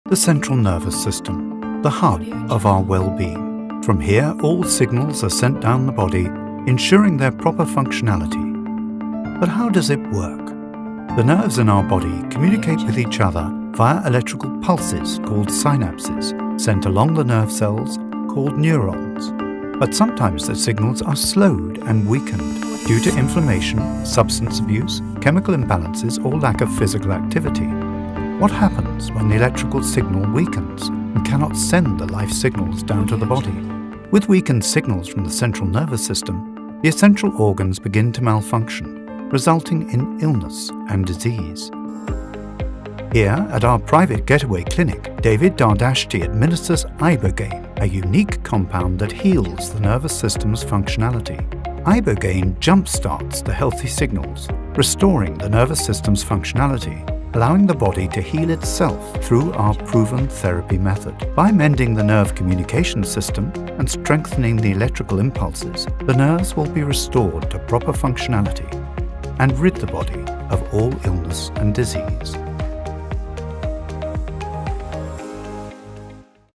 Revitalize Your Health: How Ibogaine Restores Nervous System Function
Discover how Ibogaine can rejuvenate your central nervous system and restore your health! In this video, we delve into the crucial role of the nervous system, explaining how electrical signals travel through neurons and what happens when they weaken due to inflammation or other issues. At our private clinic, David Dardashti utilizes Ibogaine to revitalize these signals, enhancing nerve communication and enabling your body to heal itself. Learn how our innovative therapy method can combat illness and disease by jumpstarting your nervous system's functionality.